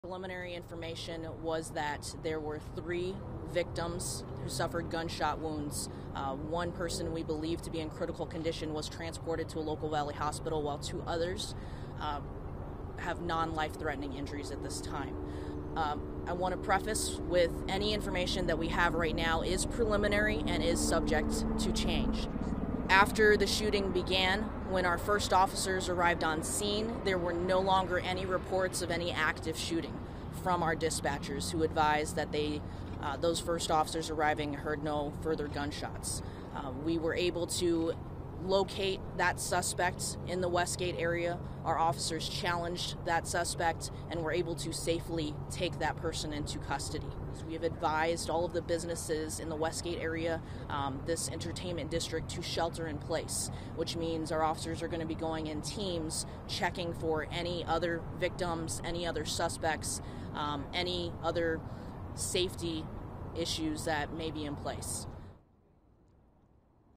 Three people wounded in Arizona shooting, police say
A suspect is in custody after three people were injured in a shooting Wednesday at the Westgate Entertainment District in Glendale, Arizona.